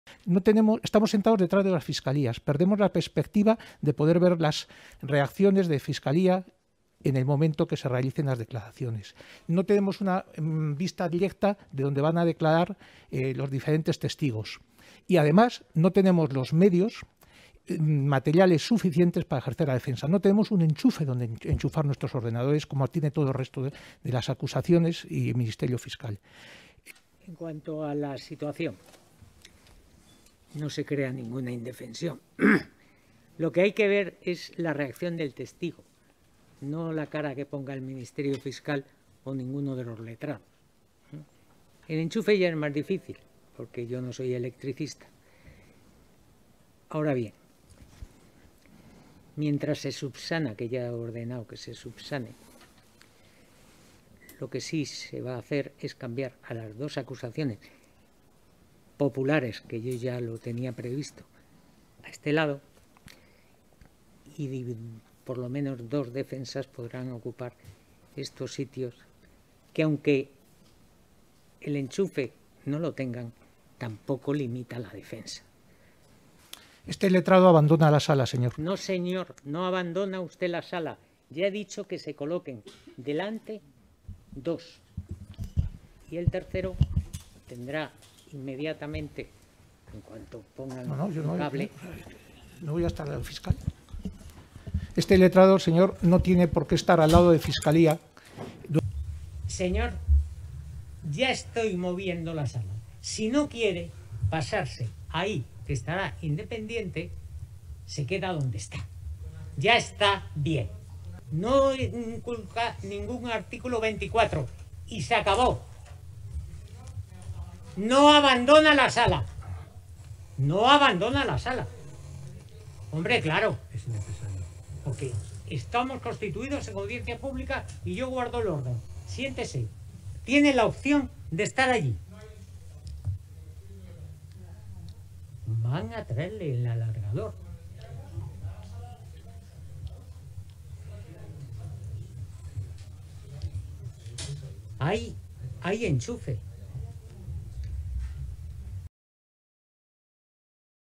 Bronca entre el juez y el abogado de uno de los acusados: “¡No abandona usted la sala!”
Fuente: Reuters